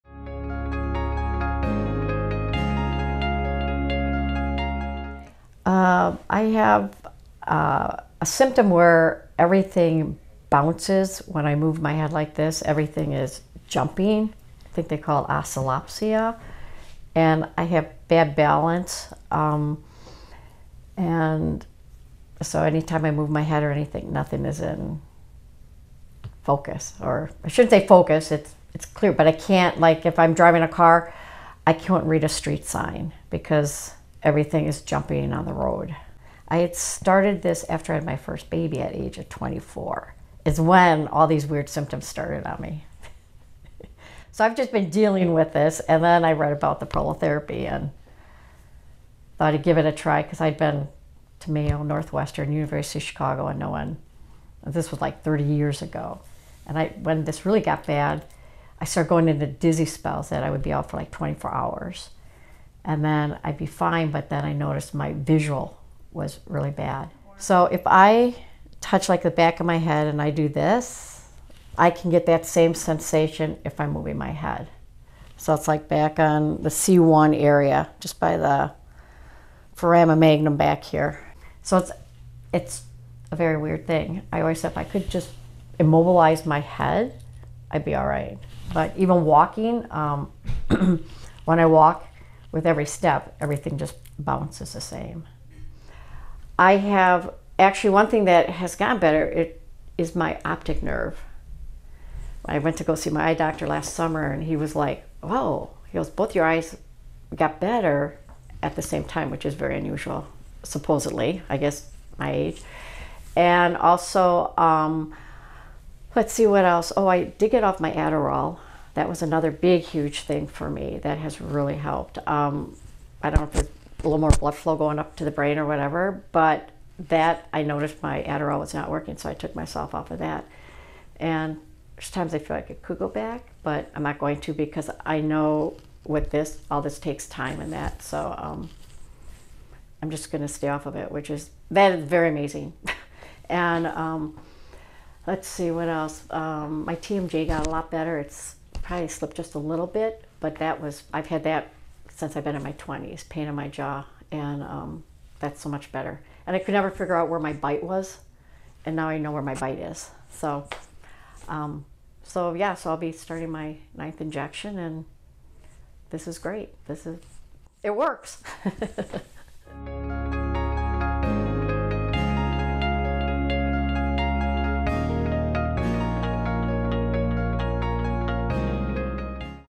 Vision and balance problems due to upper cervical instability- Debbie's Prolotherapy story
Our fabulous patient, Debbie, gives a brief history of her most interesting symptoms, including oscillopsia and dizziness, that she has experienced for 40 years, and discusses how Prolotherapy to the upper cervical spine has helped improve her symptoms and her optic nerve function, as well as eliminated her need for Adderall medication. We are happy that we've been able to help her make such significant progress after decades of trying many other specialty clinics! Way to stick to it and thanks for sharing your story, Debbie!

You can learn more about Prolotherapy, and have the Caring Medical team review your case